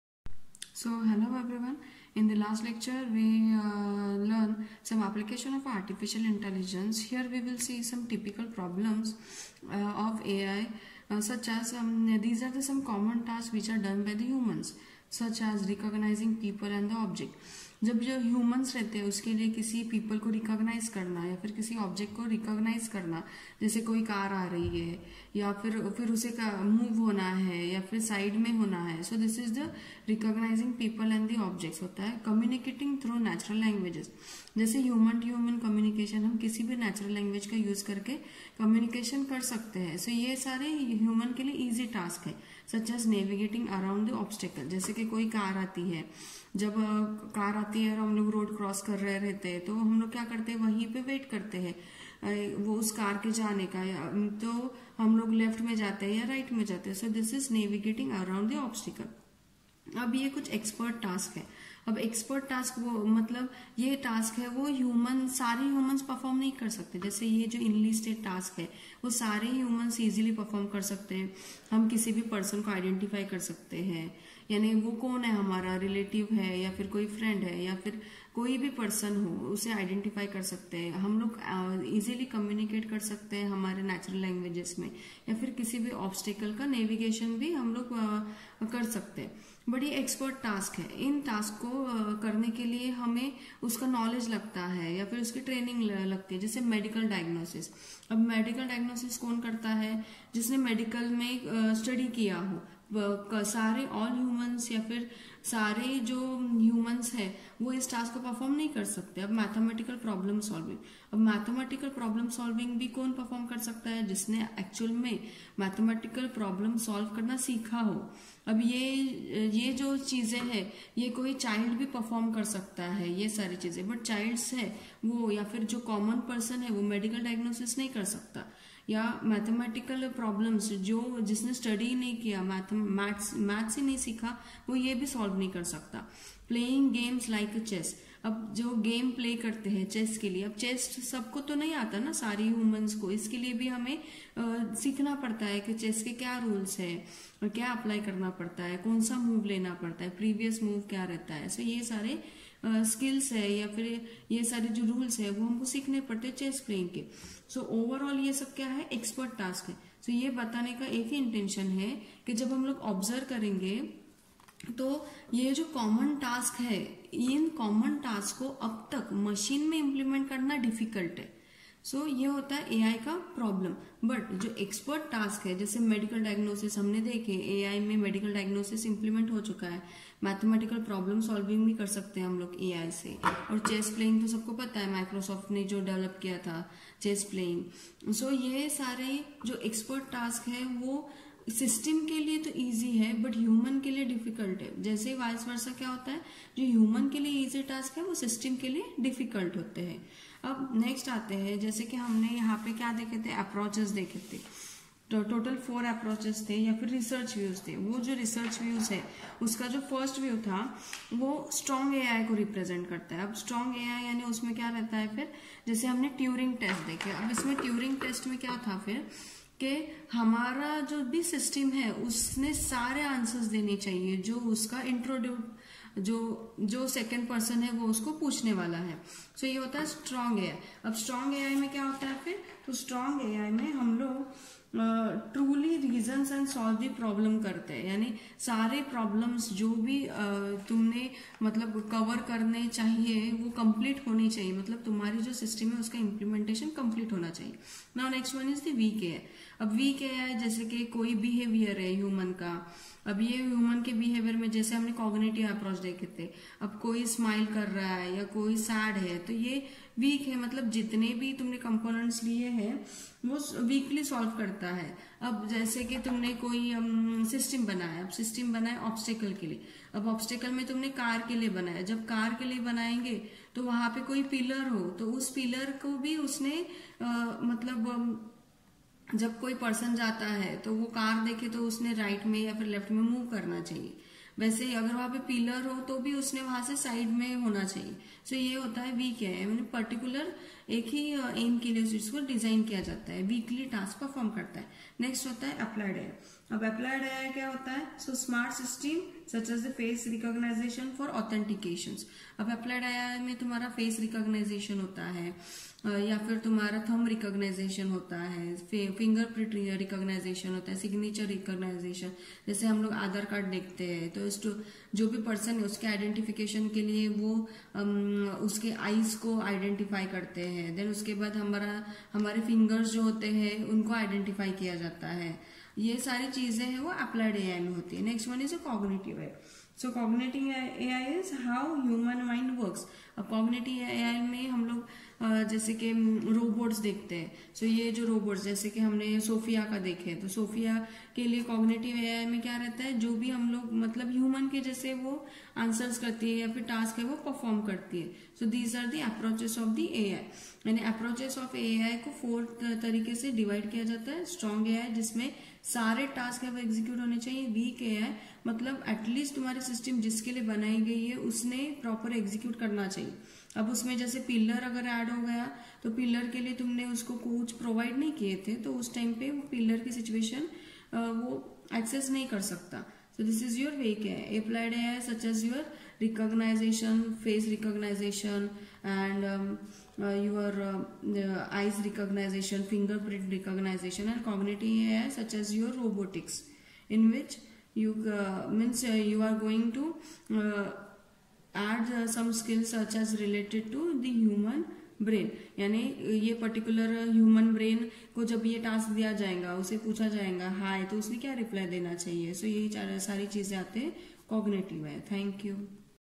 TYPICAL AI PROBLEMS
In this video we are going to see some common task and expert task done by human.